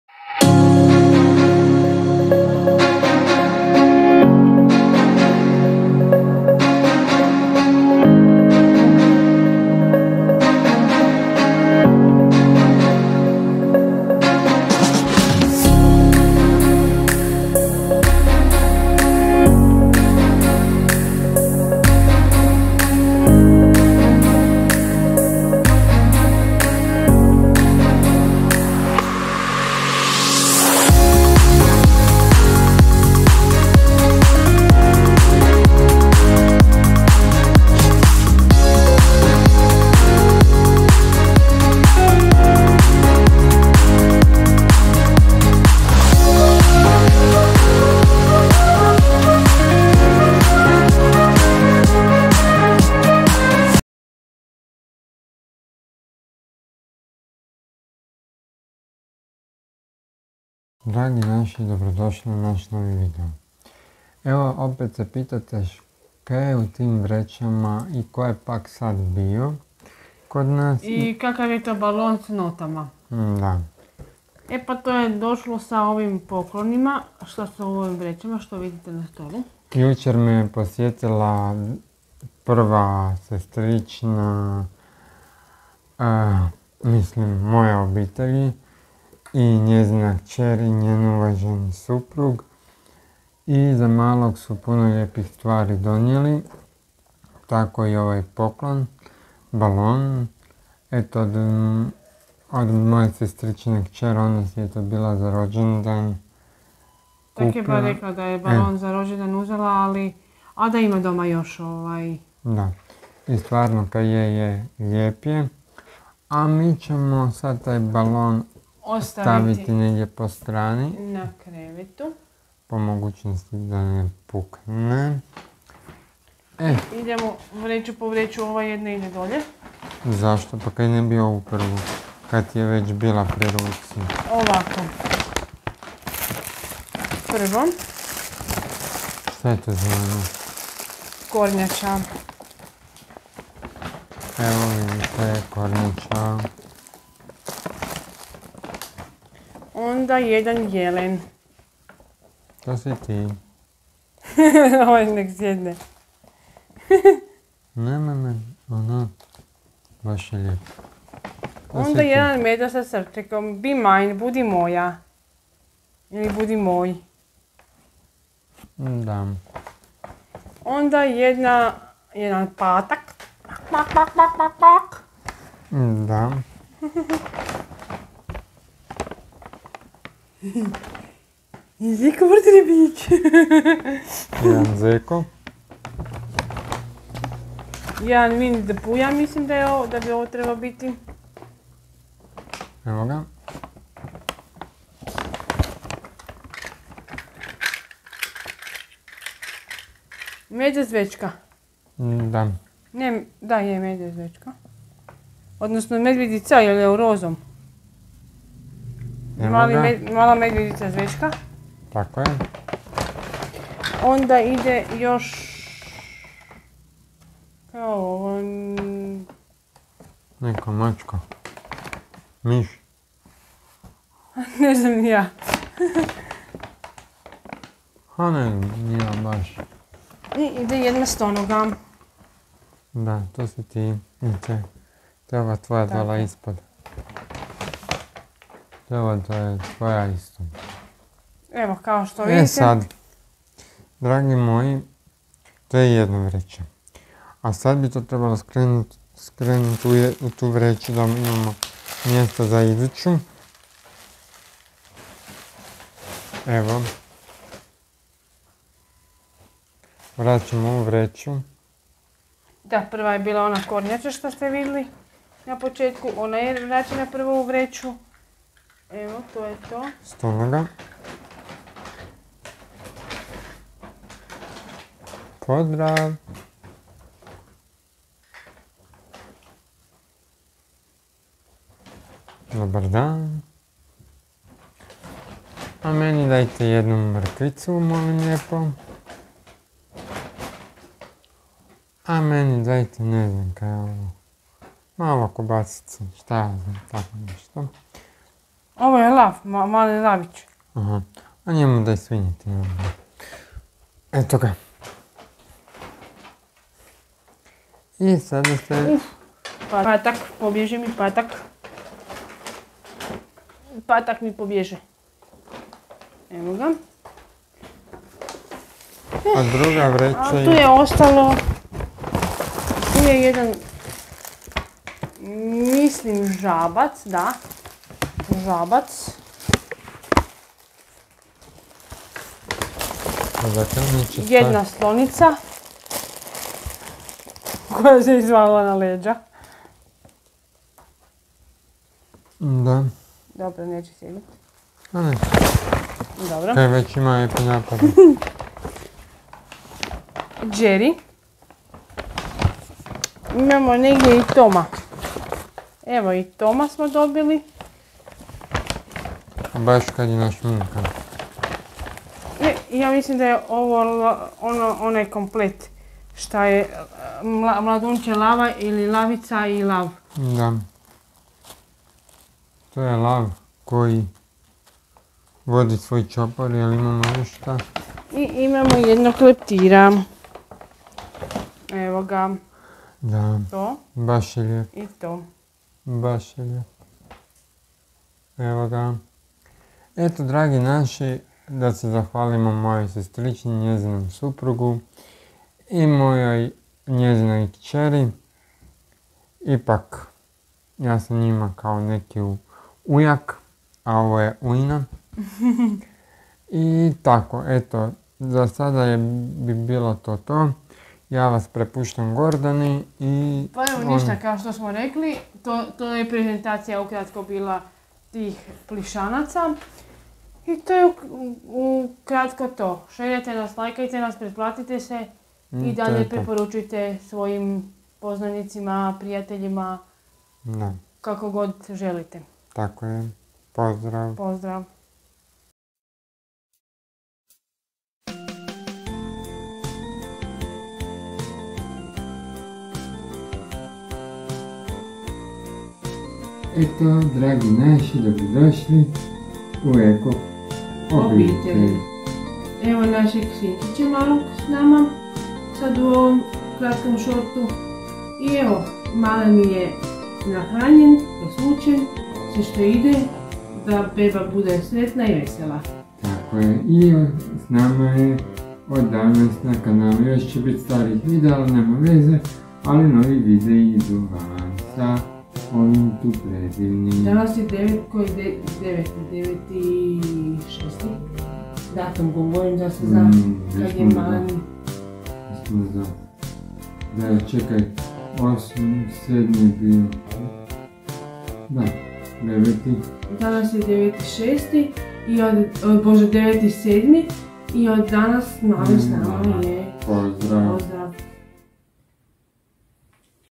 Kada te sestrična posjeti zajedno s mužem i svojom kčeri našeg sina Ivana
došla je od mojeg supruga sestrična zajedno s kceri i mužem njena kcer je donjela puno plišanih igrački i jedan velik balon  stvarno je bilo lijepo družeči se s njima u našem stanu jedva čekamo da opet navrate u posjetu